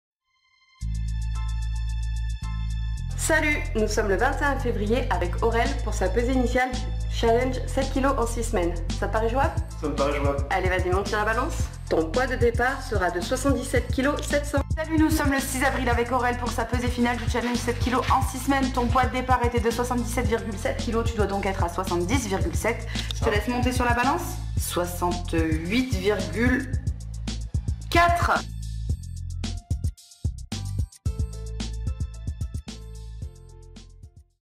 Transformation physique rapide Lyon
Aurele a perdu 9,300 kgs en 6 semaines à la Training Academy